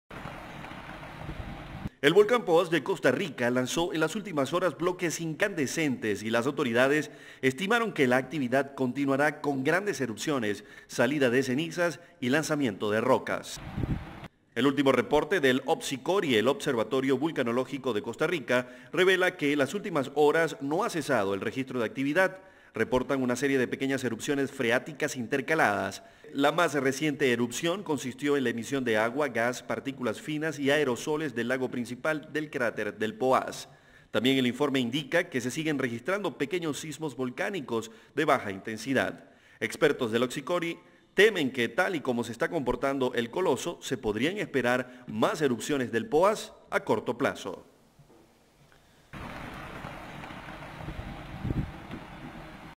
El volcán Poás sigue en actividad en Costa Rica.
Expertos del Observatorio Vulcanológico y Sismológico de Costa Rica  se mantienen en alerta, luego que la actividad del  volcán Poás  se ha hecho cada vez más cambiante.

El Poás lanzó en las últimas horas bloques incandescentes y las autoridades estimaron que la actividad continuará con "grandes" erupciones, salida de ceniza y lanzamiento de rocas.

El último reporte del OVSICORI, el Observatorio Vulcanológico de Costa Rica,  revela que las últimas horas no ha cesado el registro de actividad, reportan una serie de pequeñas erupciones freáticas intercaladas. La más reciente erupción consistió en la emisión de agua, gas, partículas finas y aerosoles del lago principal del cráter.

Expertos indica que tal y como se comporta el coloso, se podrían esperan más erupciones del Poás a corto plazo.

Gustavo Velazco/CB24